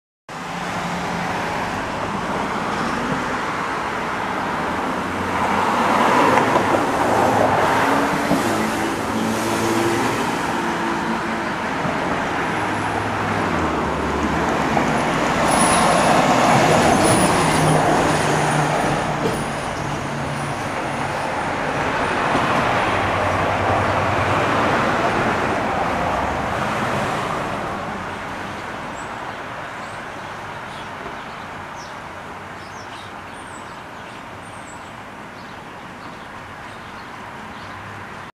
Trenton FD Engine 6
Passing by in Hamilton Township, NJ while returning to it's quarters in the city of Trenton after a food run. Rig is a 2018 KME Severe Service Pumper. A Trenton FD hazmat unit passed by minutes ago but it caught me totally off guard.